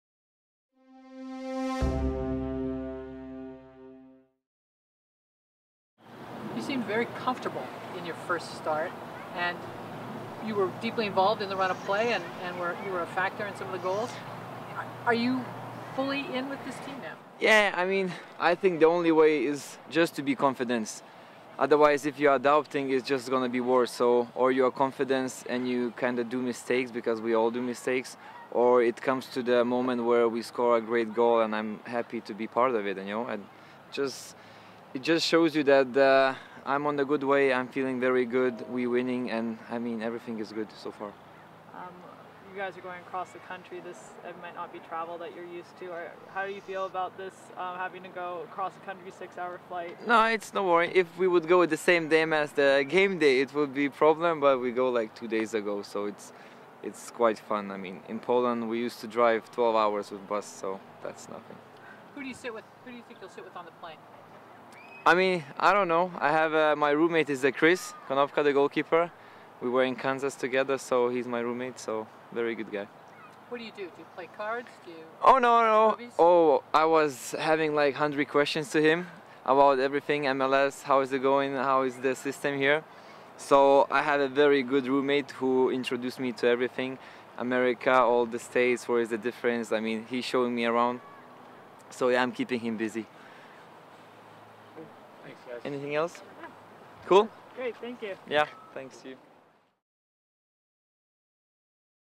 Vytas is getting to know MLS, USA
Vytas -- it&apos;s on his jersey -- spoke after Portland Timbers training on Tuesday about getting used to the team and to the travel, thanks especially to his new roommate on the road, backup goalkeeper Chris Konopka.